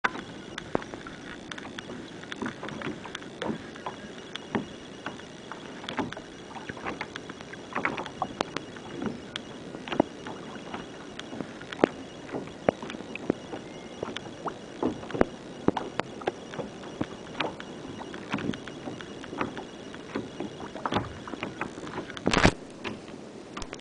Glover's Reef, Northeast Caye
In Belize, as shot from my kayak